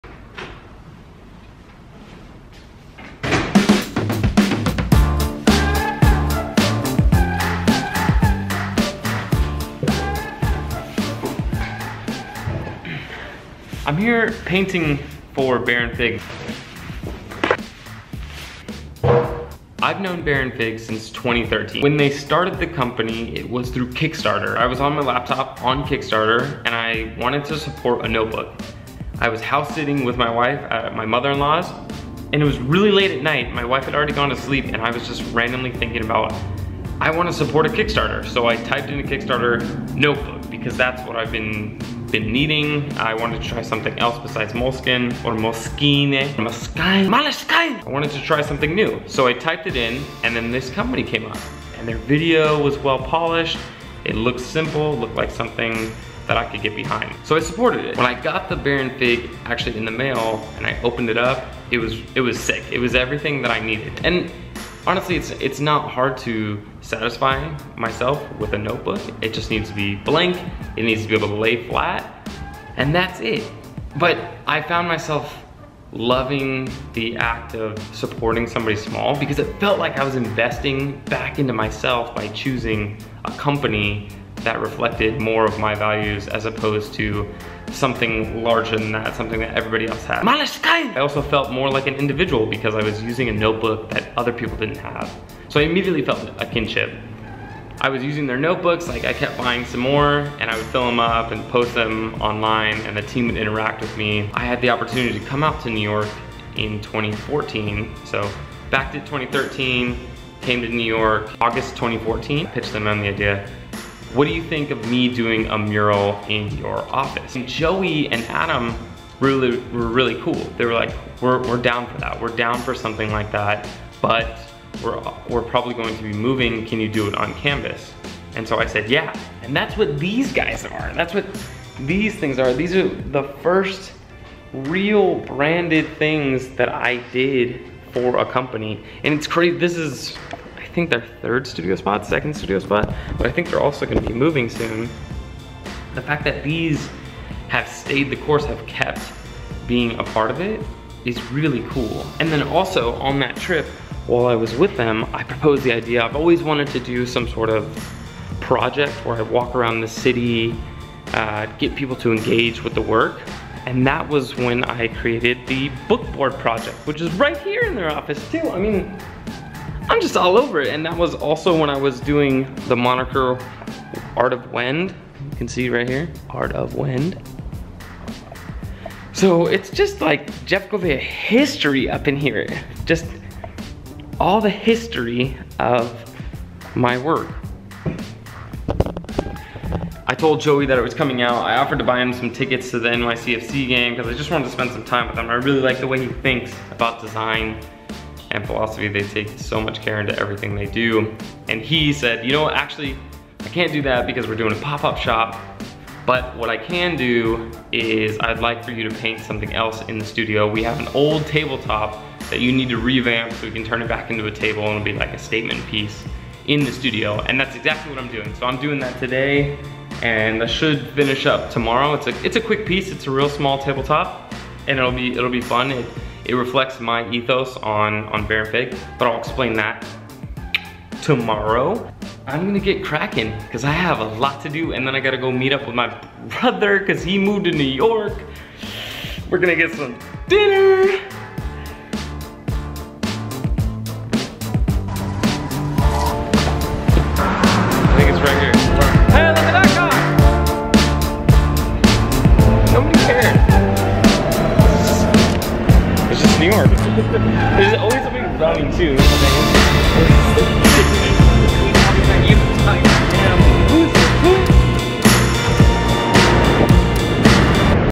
NYC VLOG: Painting for Baron Fig (Commission!)
I'm out in NYC painting for my favorite sketchbook company, Baron Fig. I'm here to finish this little commission up and explore the city for a bit. A little behind the scenes history of my career in this one too!